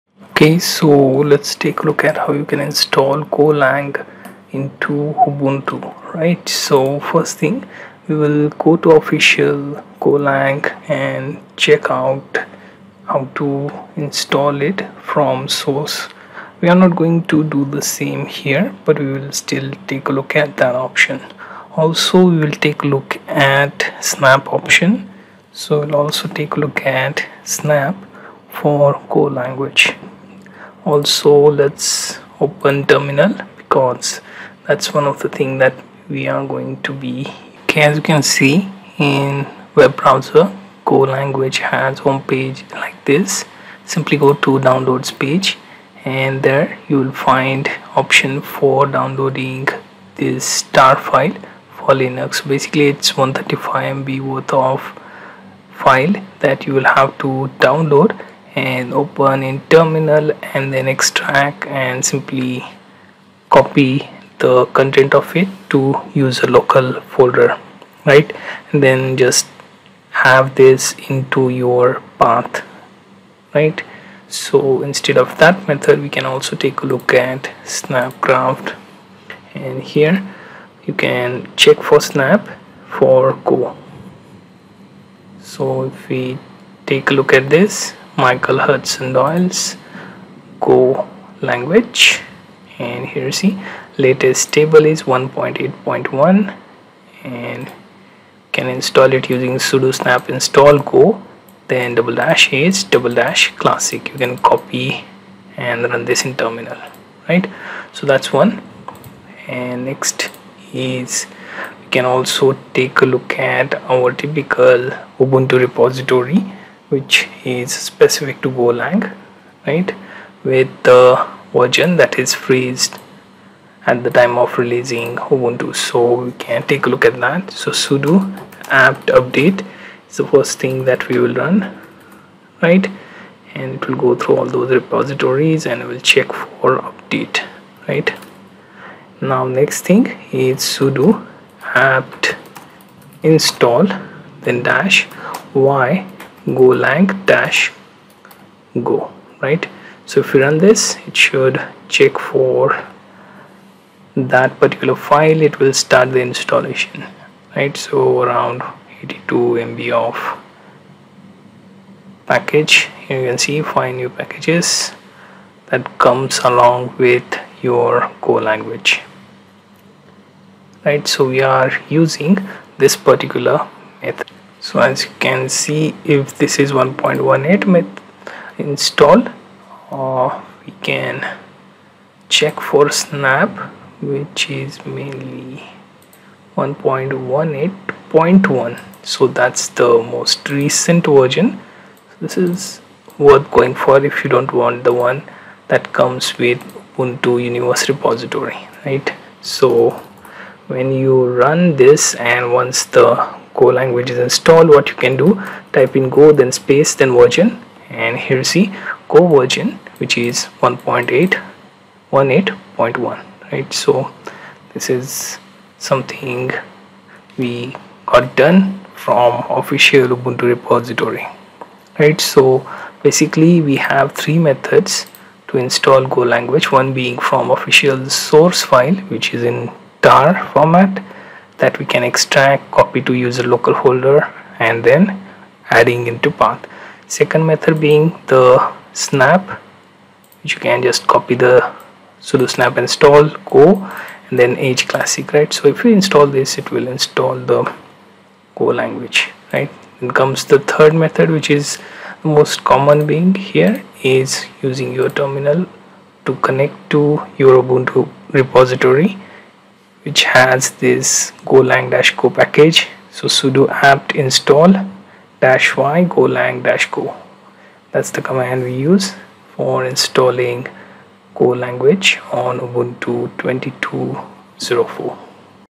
Golang - How to Install Go (Golang) Compiler on Ubuntu
Learn How to Install Go (Golang) Compiler on Ubuntu Linux. Install Go (Golang) on Ubuntu from PPA Repository. How to install Golang via Linux terminal.